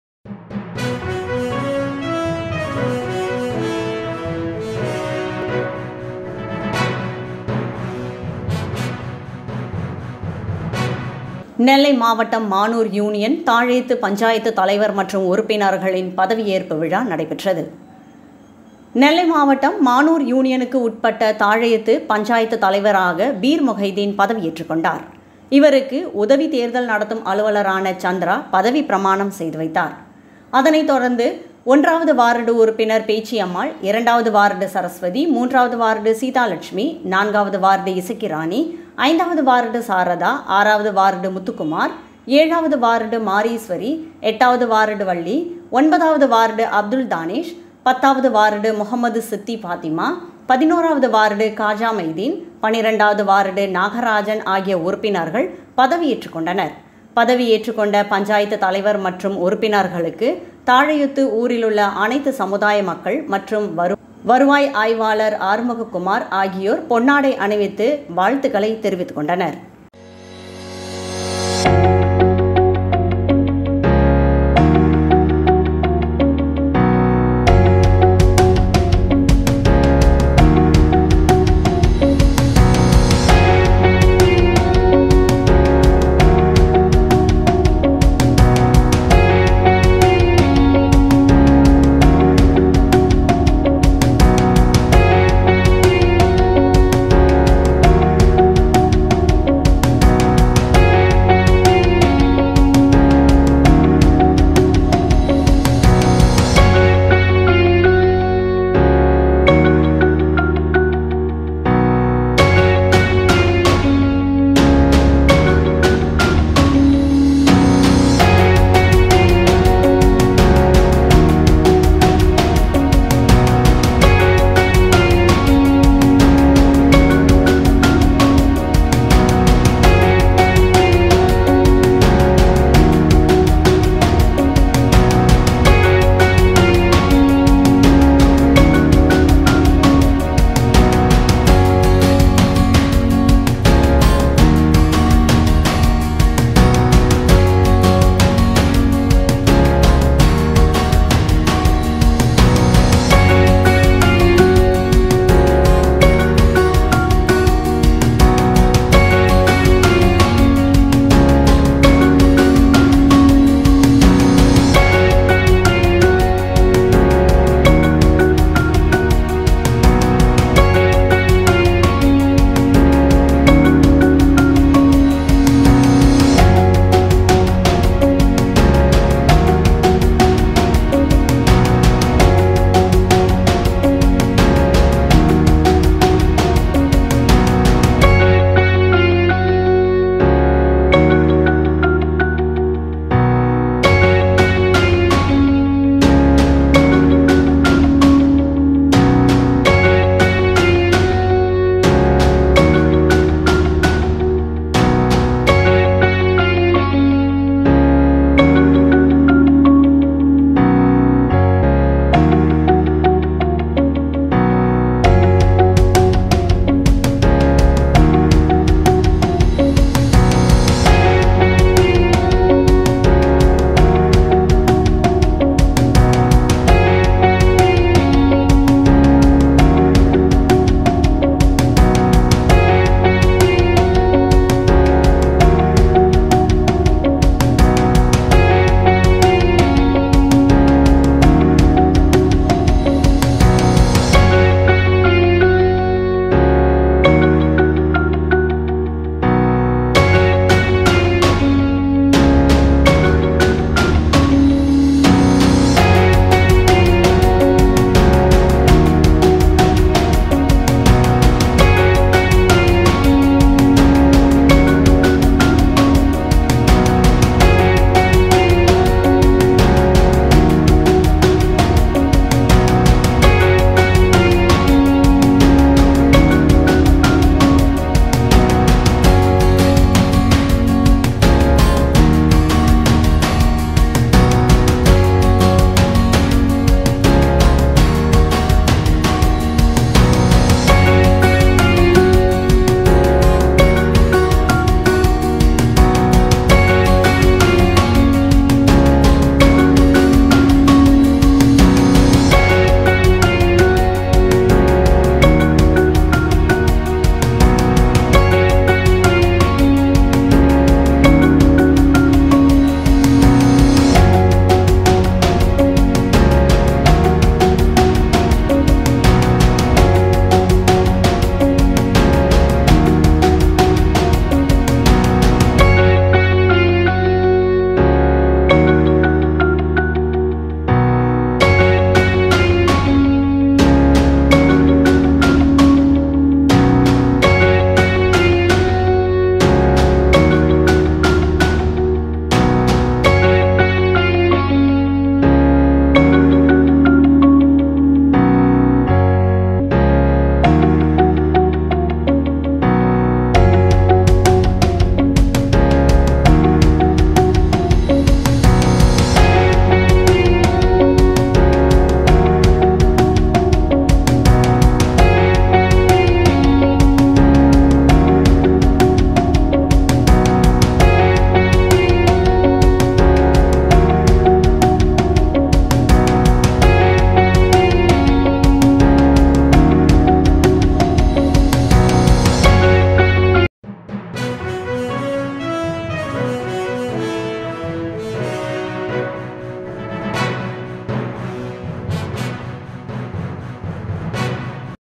நெல்லை மாவட்டம் மானூர் யூனியன், தாழையூத்து பஞ்சாயத்து தலைவர் மற்றும் உறுப்பினர்கள் பதவியேற்பு விழா
நெல்லை மாவட்டம் மானூர் யூனியனுக்கு உட்பட்ட தாழையூத்து பஞ்சாயத்து தலைவராக பீர்முகைதீன் பதவி ஏற்றுக்கொண்டார். இவருக்கு  உதவி தேர்தல் நடத்தும் அலுவலரான சந்திரா பதவி பிரமாணம் செய்து வைத்தார். அதைதொடர்ந்து 1-வார்டு உறுப்பினர் பேச்சியம்மாள் 2வது வார்டு சரஸ்வதி 3வது -வார்டு சீதாலெட்சுமி, 4வது வார்டு இசக்கி ராணி, 5வது வார்டு சாரதா, 6வது வார்டு முத்துகுமார், 7வது வார்டு மாரீஸ்வரி, 8 வது வார்டு வள்ளி, 9வது வார்டு அப்துல் தானிஷ், 10வது வார்டு முகம்மது சித்தி பாத்திமா, 11வது வார்டு காஜாமைதீன், 12வது வார்டு நாகராஜன் ஆகிய உறுப்பினர்கள் பதவி ஏற்றுக்கொண்டனர். பதவி ஏற்றுக்கொண்ட பஞ்சாயத்து தலைவர் மற்றும் உறுப்பினர்களுக்கு தாழையூத்து ஊரிலுள்ள அனைத்து சமுதாய மக்கள்,மற்றும் வருவாய் ஆய்வாளர் ஆறுமுக குமார், ஆகியோர் பொன்னாடை அணிவித்து வாழ்த்துக்களை தெரிவித்துக் கொண்டனர்.